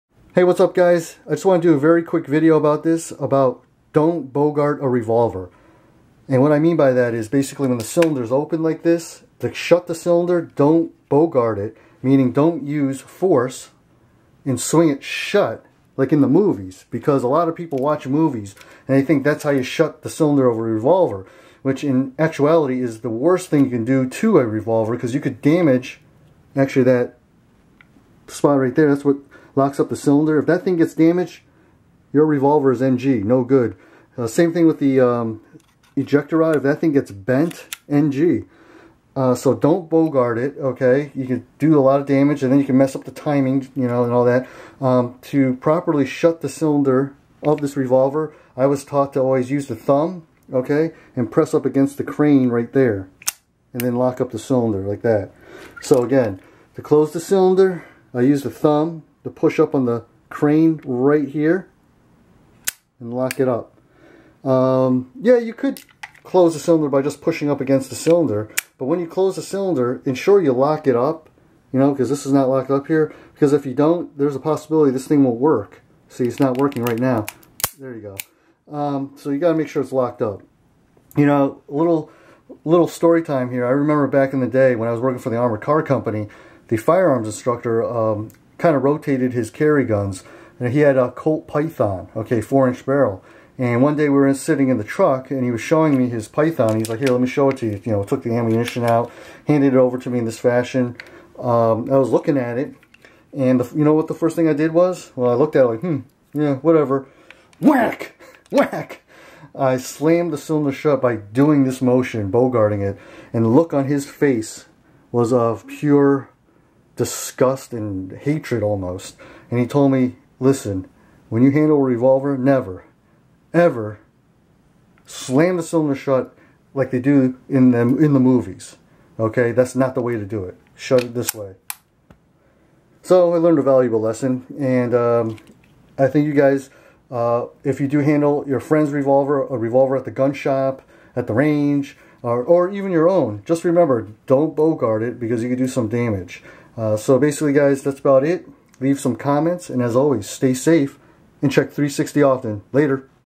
Don't Bogart A Revolver
Quick vid about closing a cylinder on a revolver.

Stay Safe and Check 360 Often!